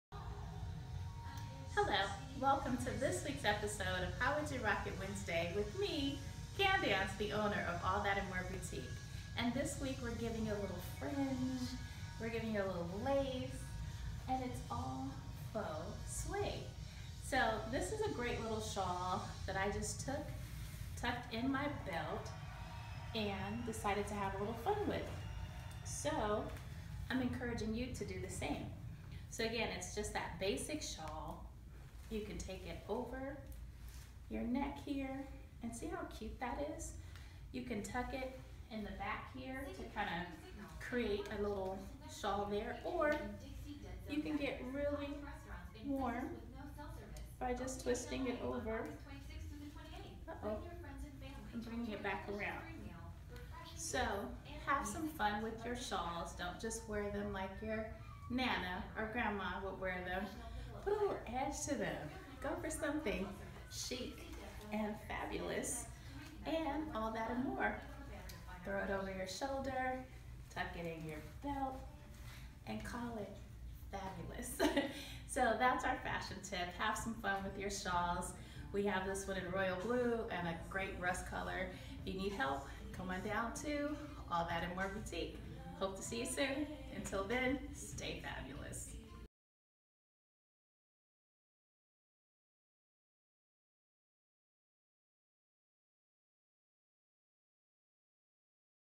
How to wear Fringe Shawl this Fall!
w How to Wear a Fringe Shawl this fall! This shawl can pair over everything. Pair it with an all black outfit.  Wrap this shawl various ways to accessorize your outfit.  Wear it long, short, or diagonal. It's just that simple. All That & MORE boutique 10754 Jefferson Blvd. Enjoy wearing the animal prints this summer!